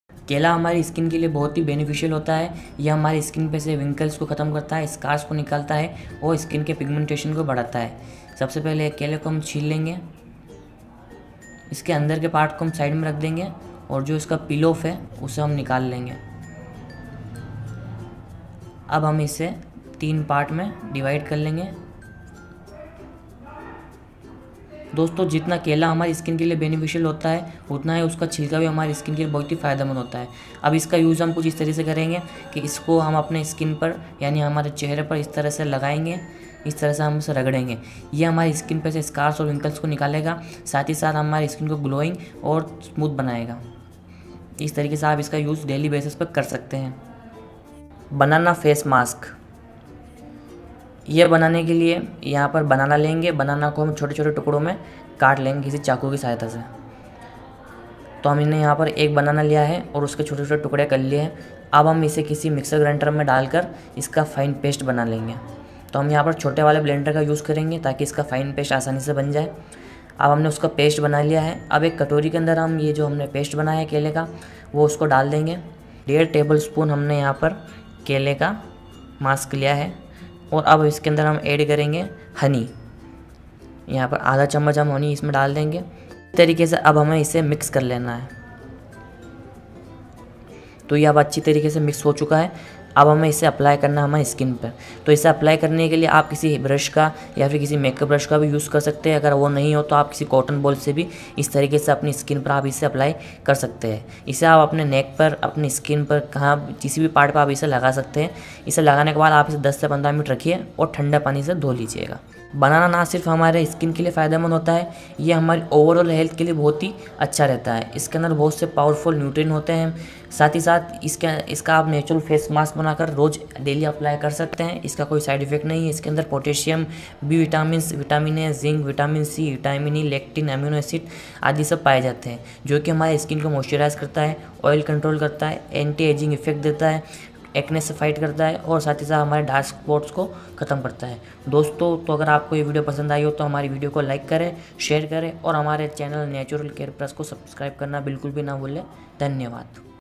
सिर्फ एक बार रगड़ने से पिंपल्स गायब हो गए। Banana peel for remove pimples, banana peel face mask
Today i will make banana peel mask and benefits of banana peel for healthy and glowing skin

Like l Share l Comment